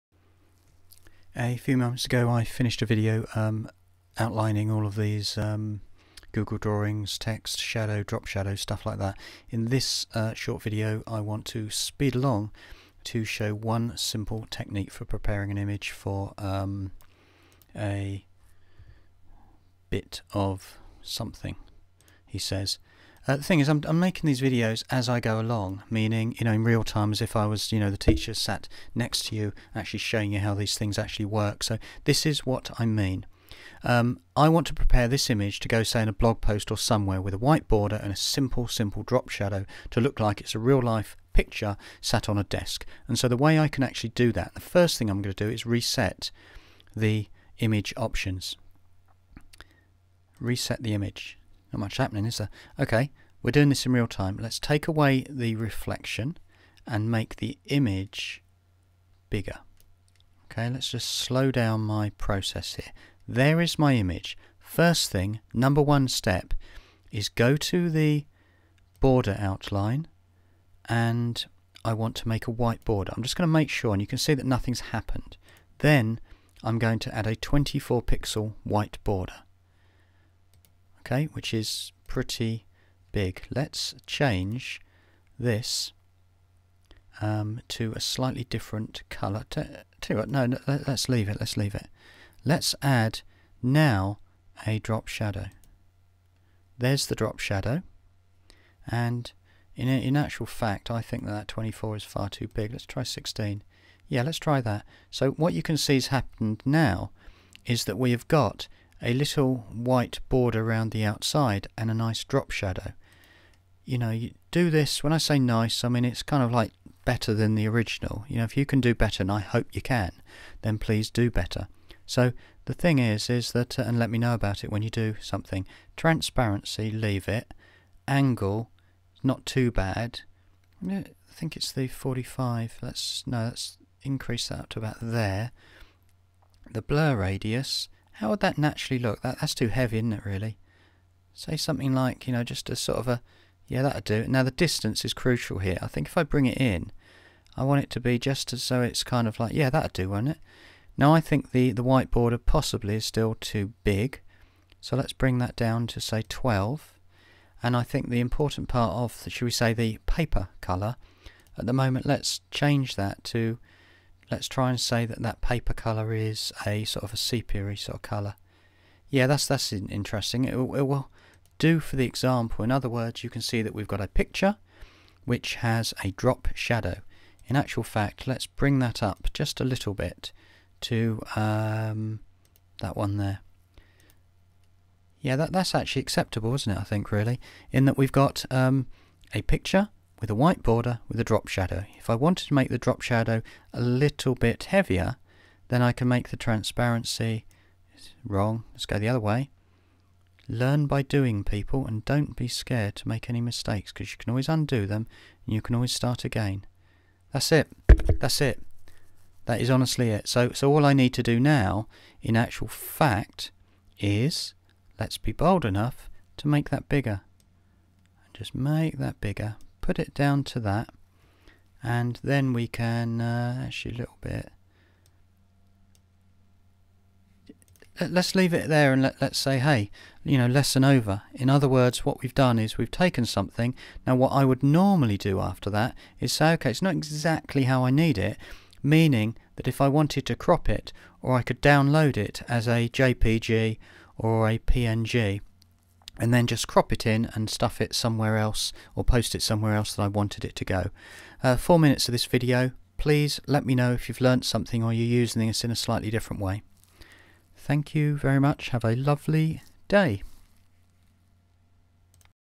Google Drawings simple image white border with drop shadow
Want a solid technique for a white border and a drop shadow - here's how.
Please play around with these features and let me know how you use them and i'd be delighted to see the results!

previous video 'Google Drawings image shape text format drop shadow reflection